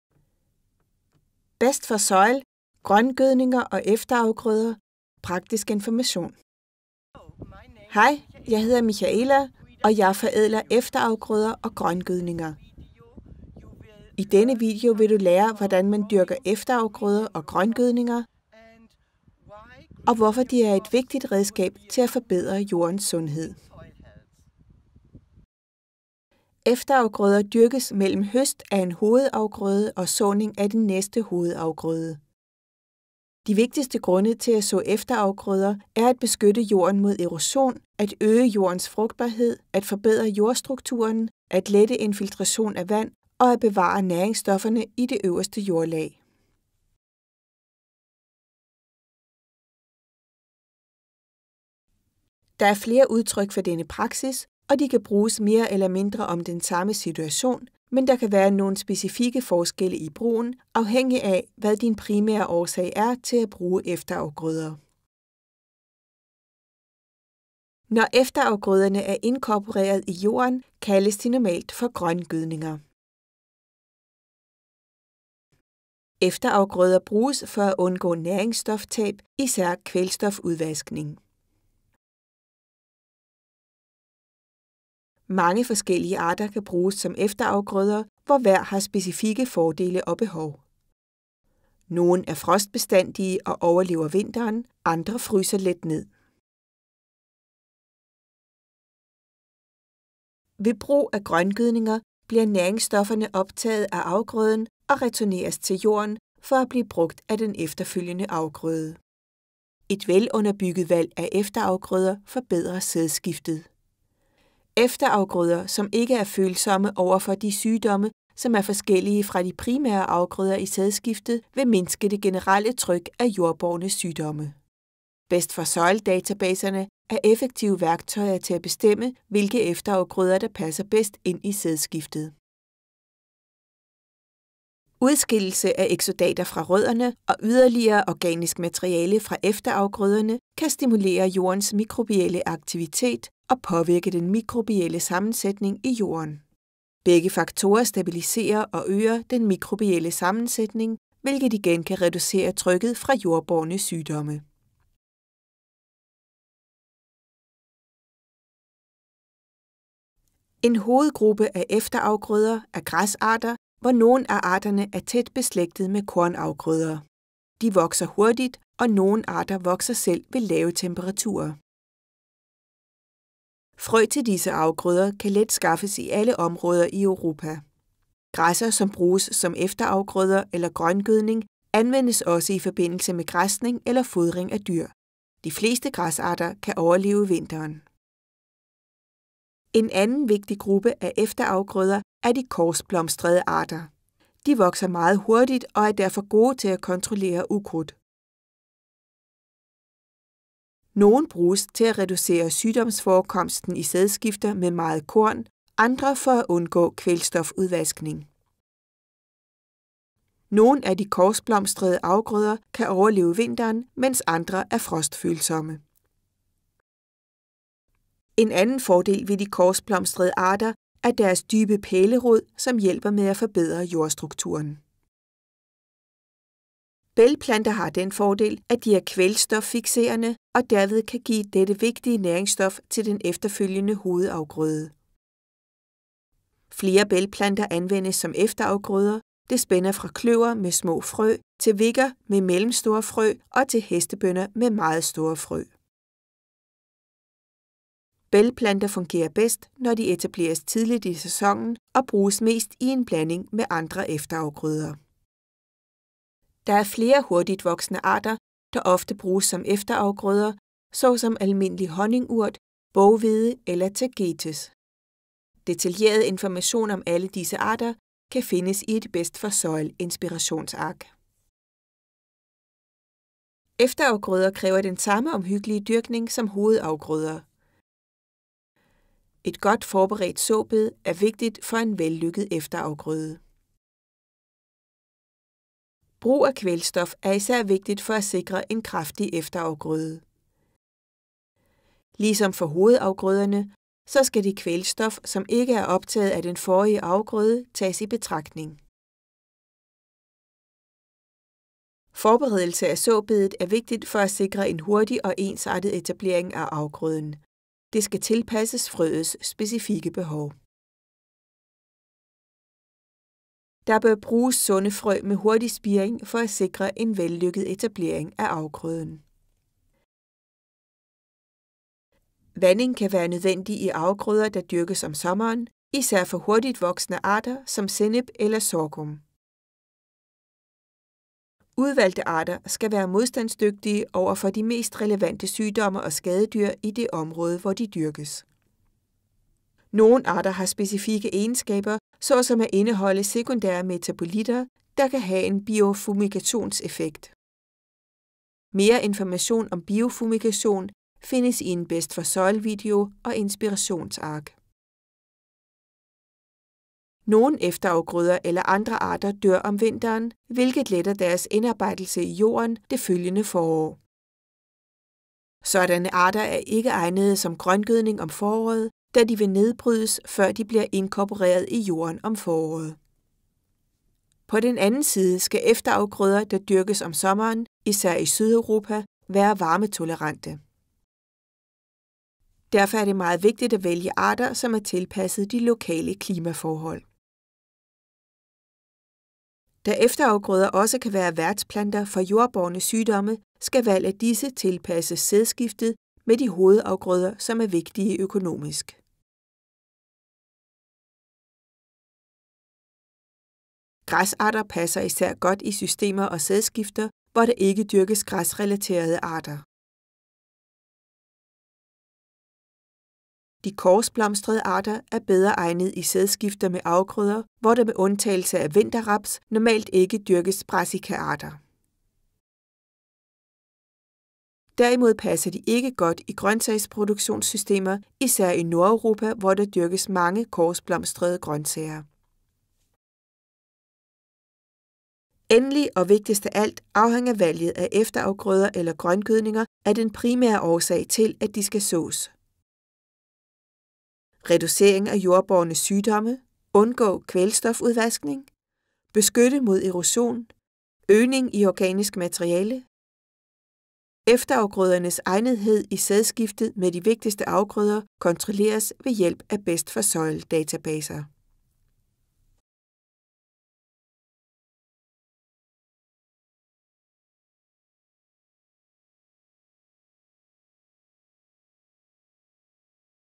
Grøngødning & Efterafgrøder – Praktisk Information
Anvendelsen af efterafgrøder og grøngødning er en af metoderne til ‘ bedste praksis’ som fremmes af Best4Soil. De forskellige aspekter af denne naturlige metode til bevarelse og forbedring af jordens sundhed præsenteres i denne video.